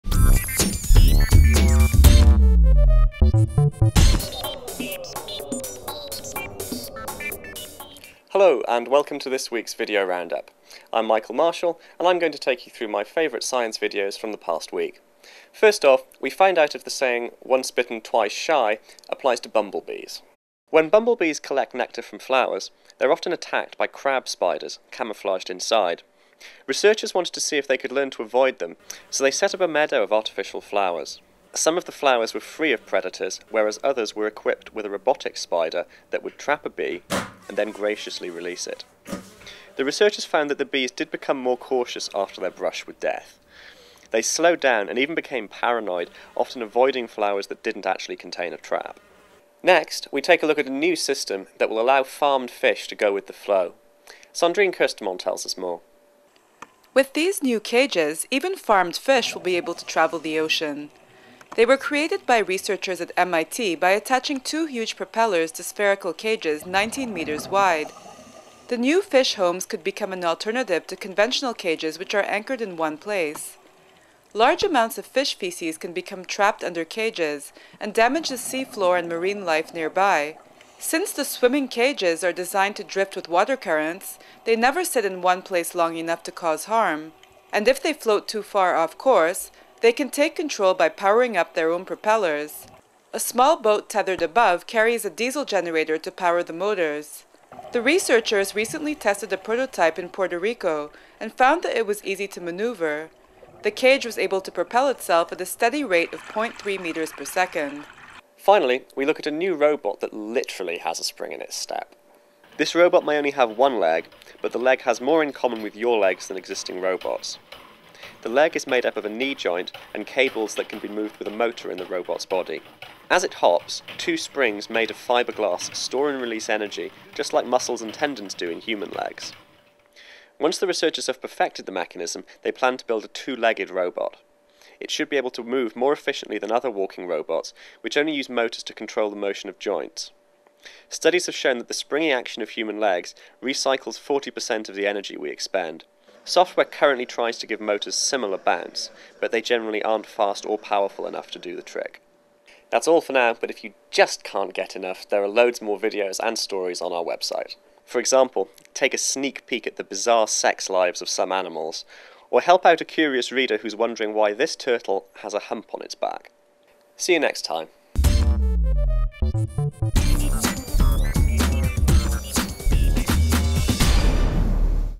New Scientist video round-up - September 5, 2008
Learn about a new springy design for robotic legs: